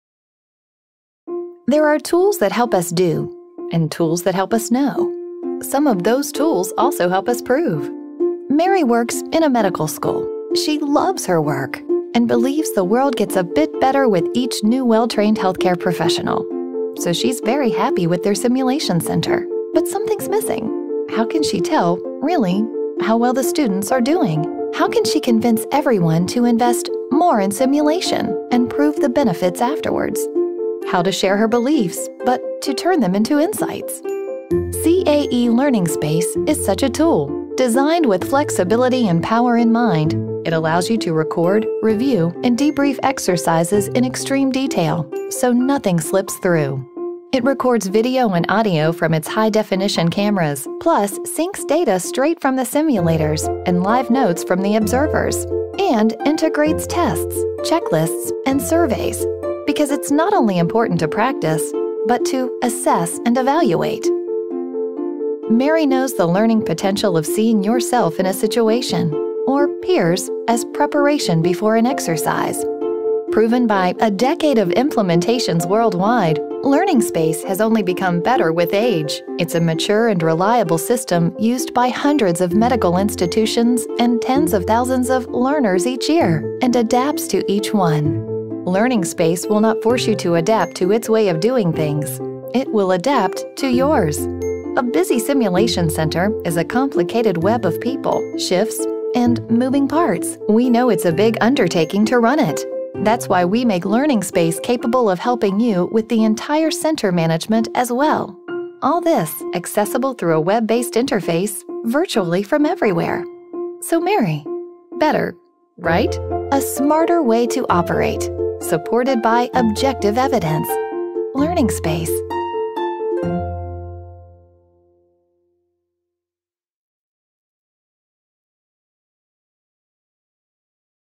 CAE Healthcare: LearningSpace Audiovisual System
Designed to connect simulation-based learning environments with the leading management and performance assessment tools for healthcare education, LearningSpace helps to manage all aspects of healthcare learning.